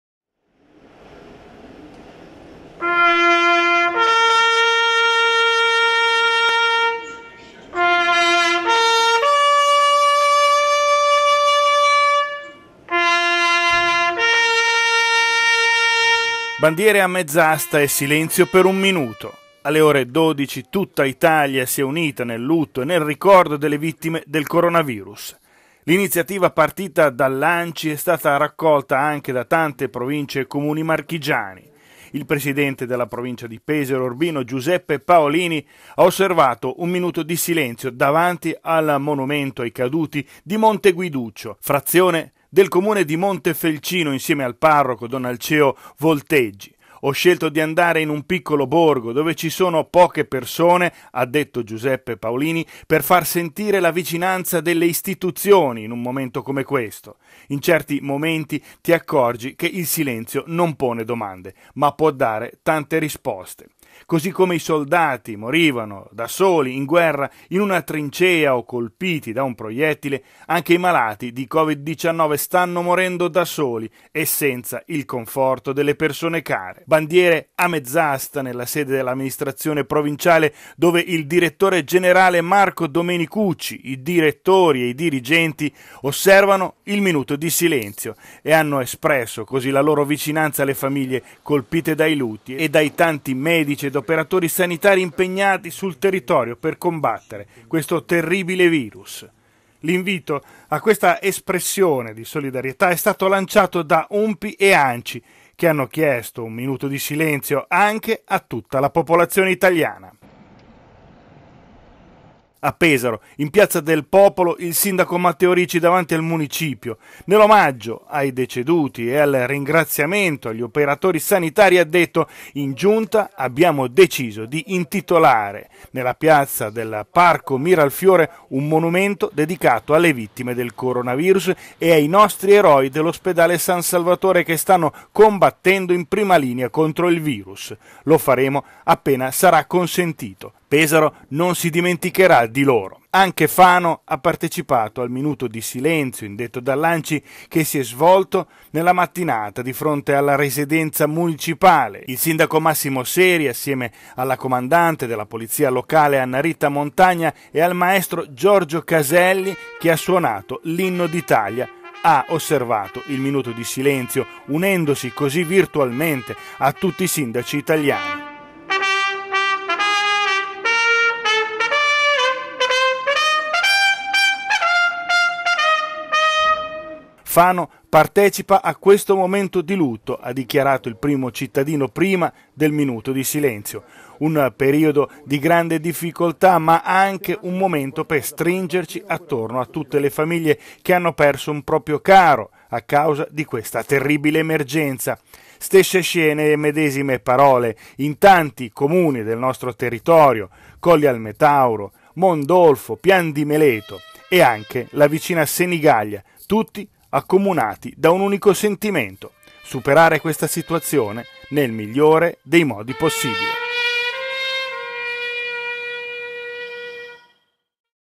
Un minuto di silenzio per le vittime Covid-19
Martedì davanti ai comuni d’Italia, si è tenuto un minuto di silenzio e bandiere a mezz'asta per ricordare tutte le vittime del Coronavirus.